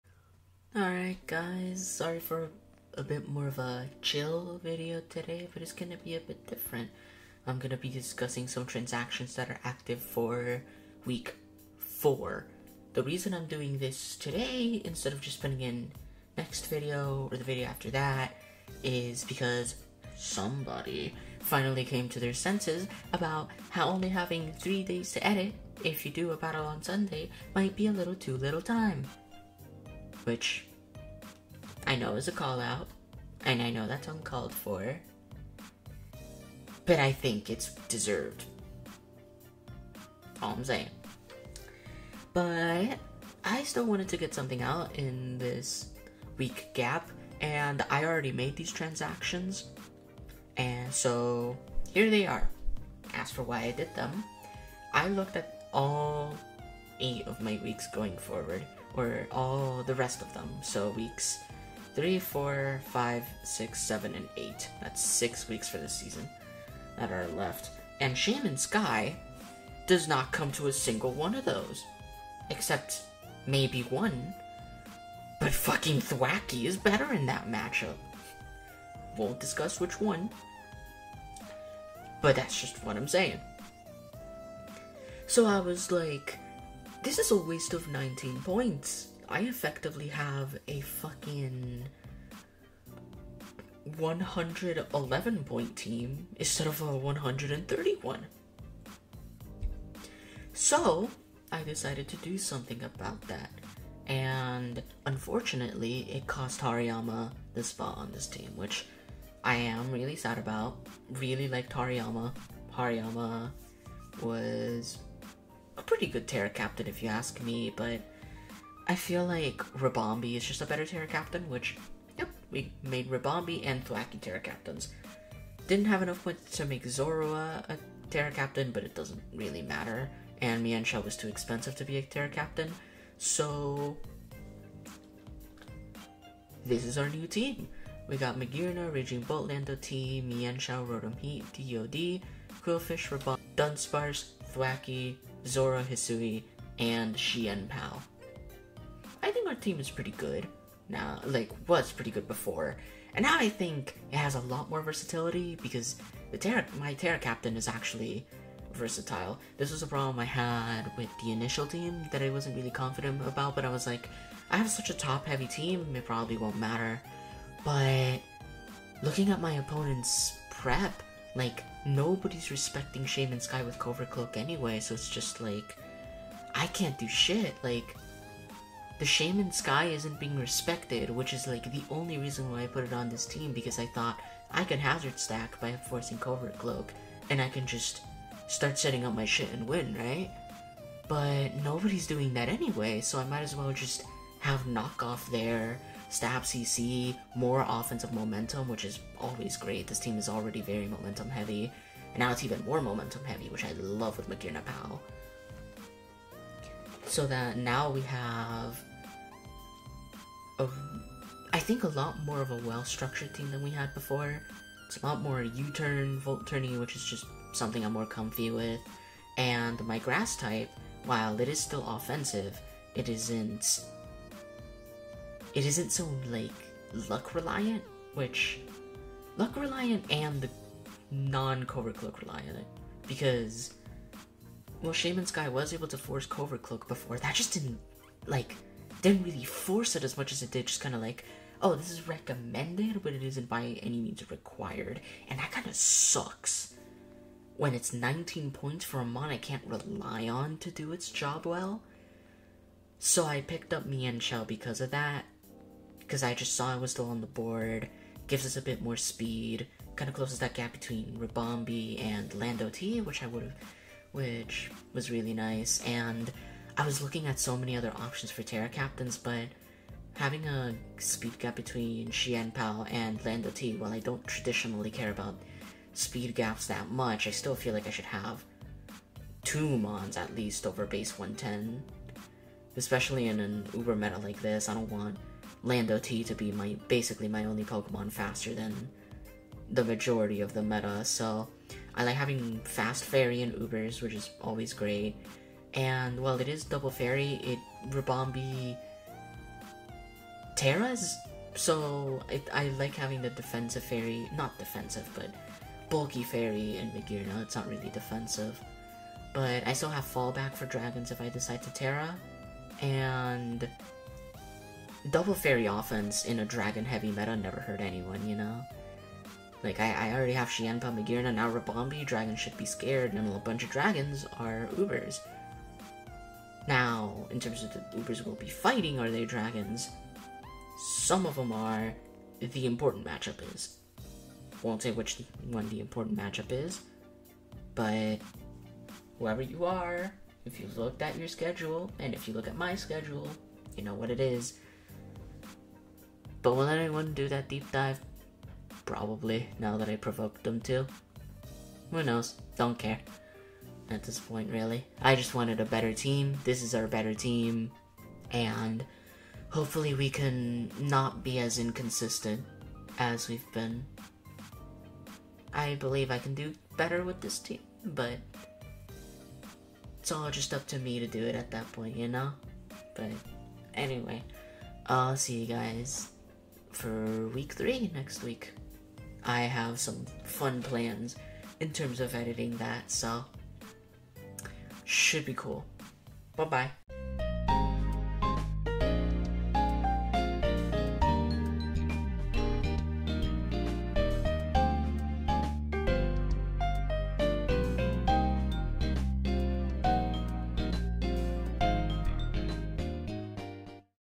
! TRANSACTIONS INCOMING ! I UPA Transaction Break :D
Dunsparce takes on the world of legendaries like Lunala and Terapagos, can the little guy hold up?

Make sure to check out the UPA Coaches!